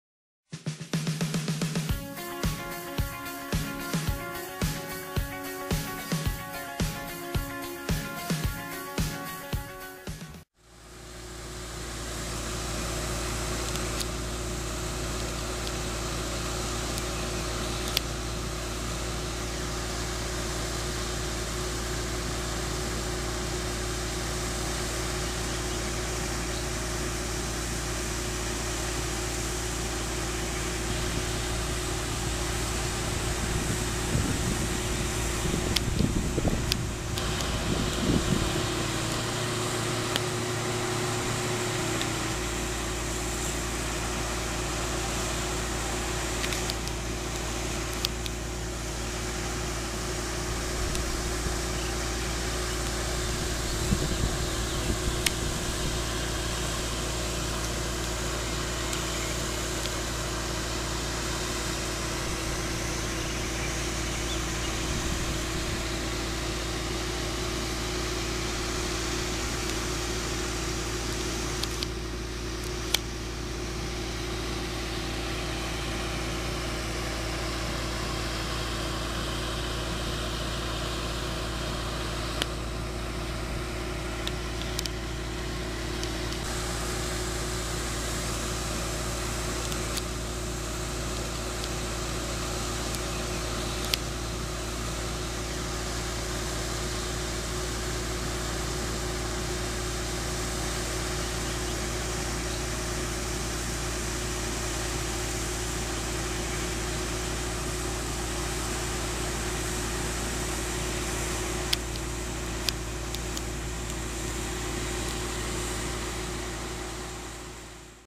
irrigazione barbatelle 2014
Irrigazione di soccorso con pompa a pioggia, di vivaio viticolo dato il protrarsi della anomala siccità in corso!